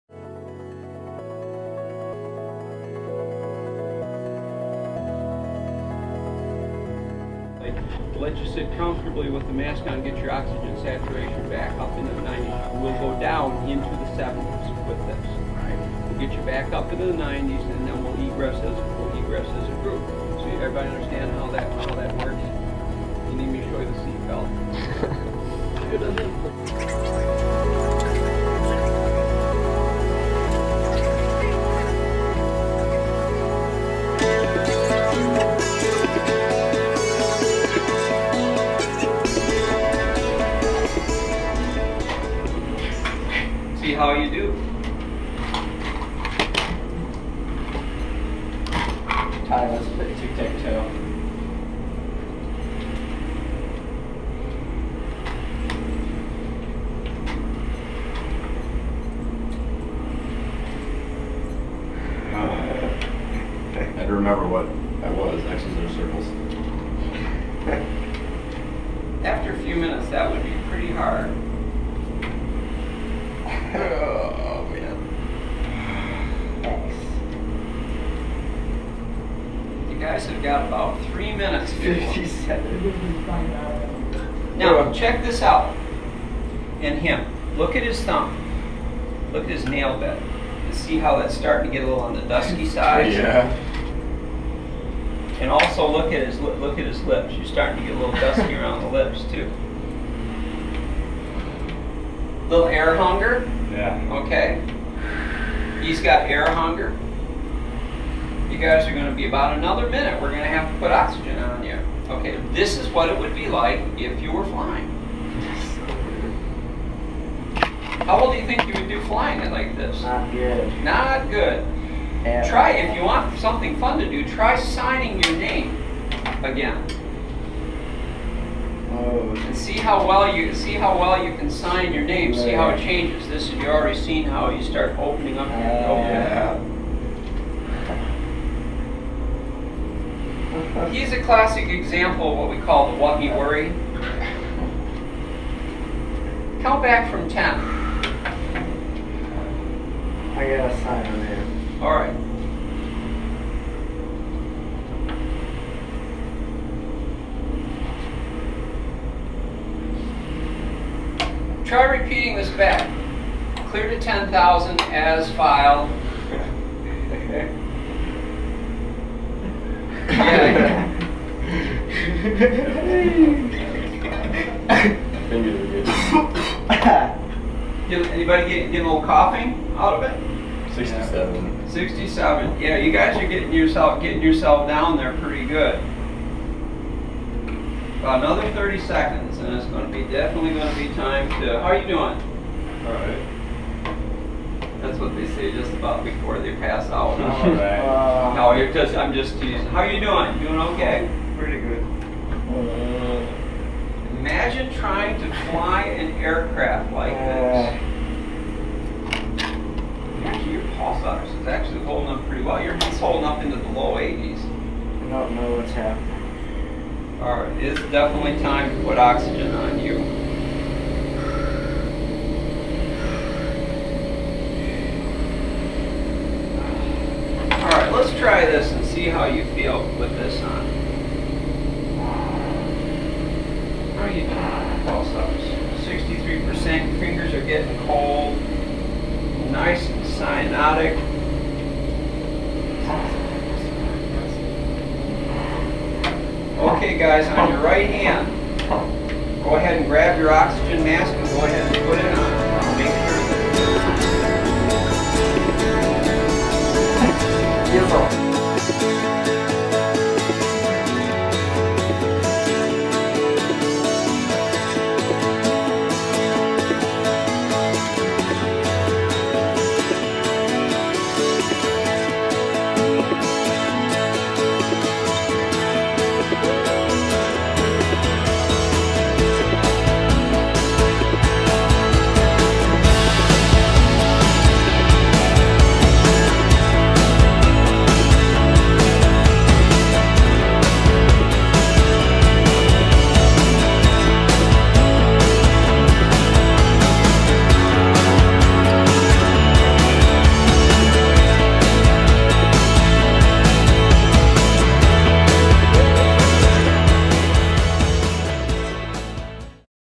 WMU Aviation Normobaric Chamber: Hypoxia Training
This is part of my flight physiology class at Western Michigan University. Simulated 30,000 feet in normobaric chamber. Filmed on a GoPro. Here is a link if you are interested in WMU.
I have enjoyed to program immensely since starting in the Fall of 2012.